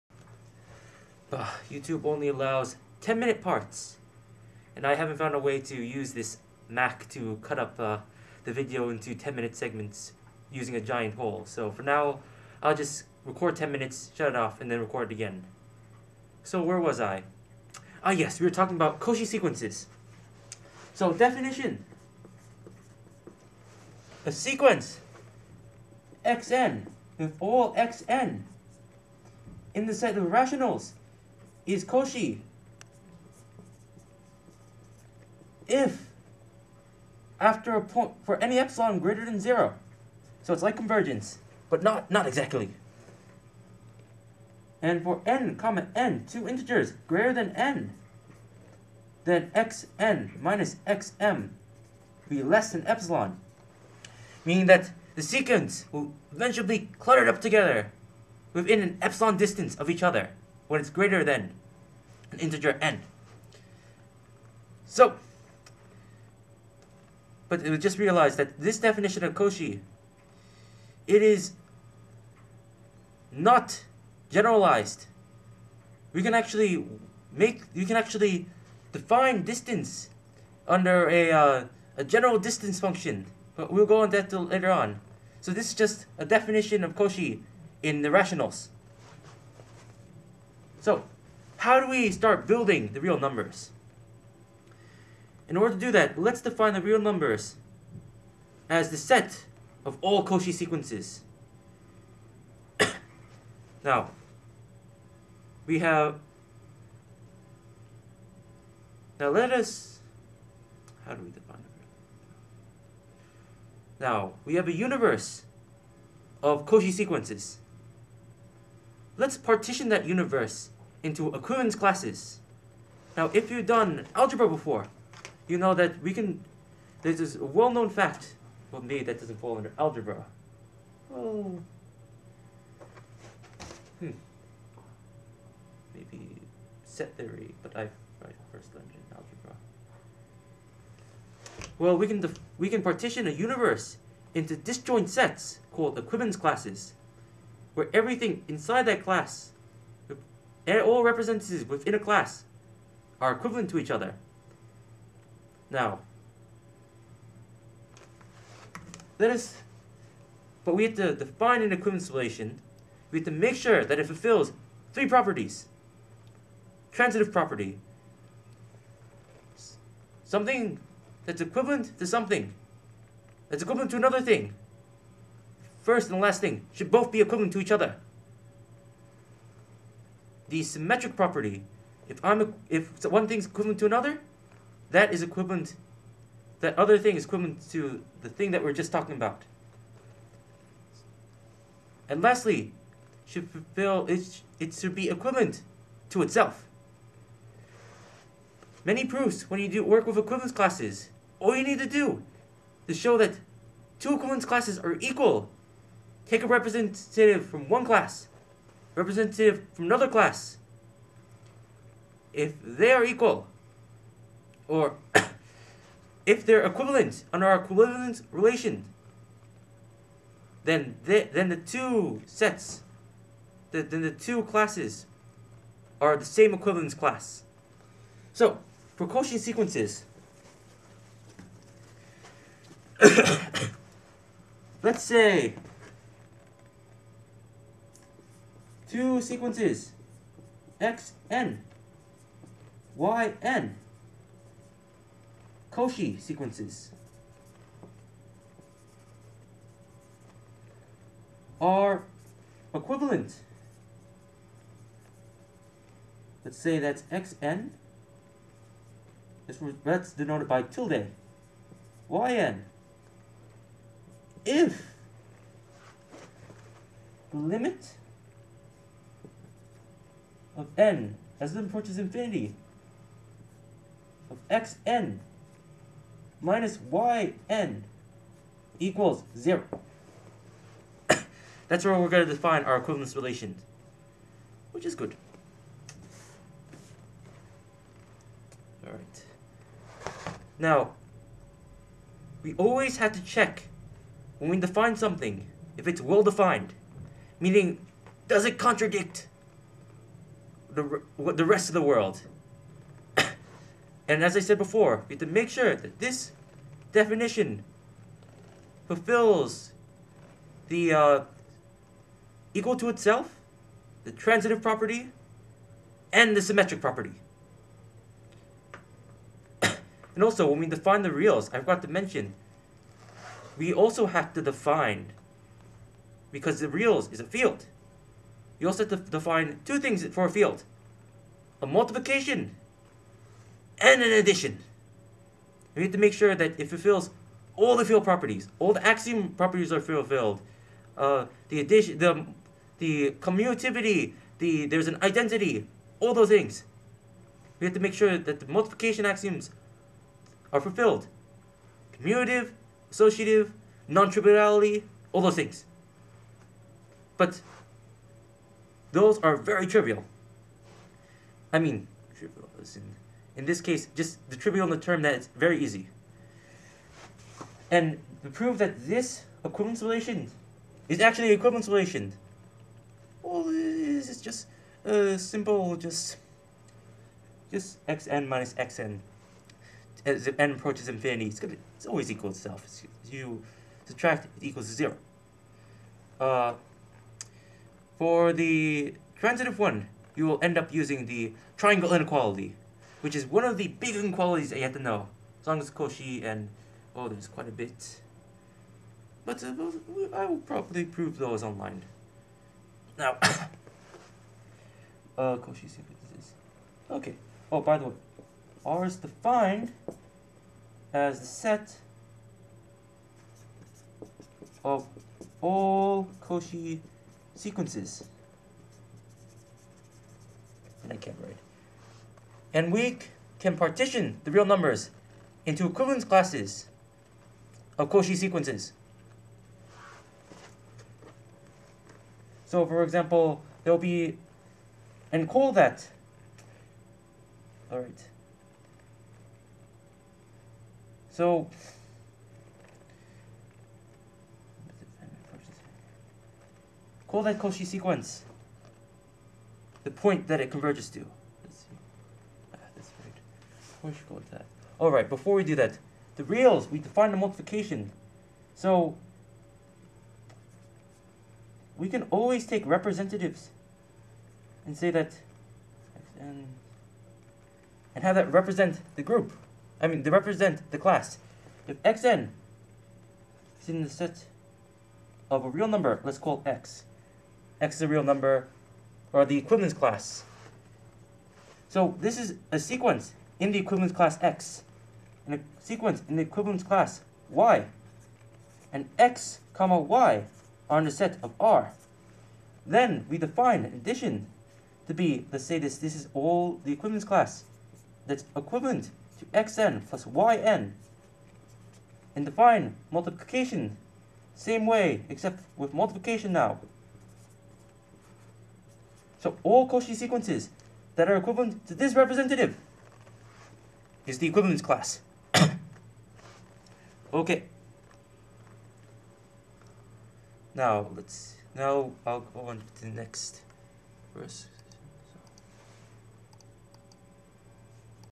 Math Ninja : Fundamental Concepts of Analysis Lecture 1-2
Short first lecture Test: Real Numbers as the Set of All Cauchy Sequences, Equivalence relation defined, definitions of Converge Short first lecture Test: Real Numbers as the Set of All Cauchy Sequences, Equivalence relation defined, definitions of Convergence, Cauchy Sequence.

Next Lecture: Proof of completeness

Will Try and find a better board.